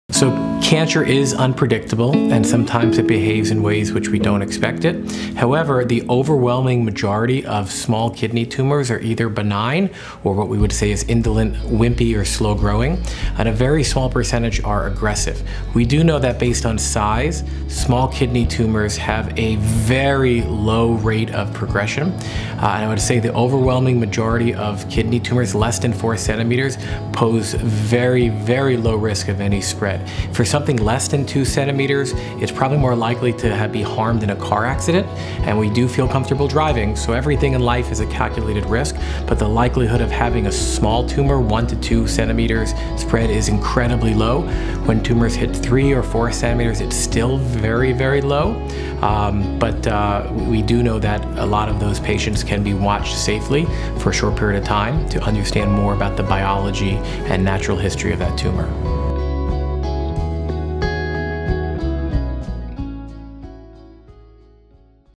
Can a small renal mass spread to other parts of the body?
Dr. Brian Shuch, Director of the Kidney Cancer Program, UCLA

for more information and resources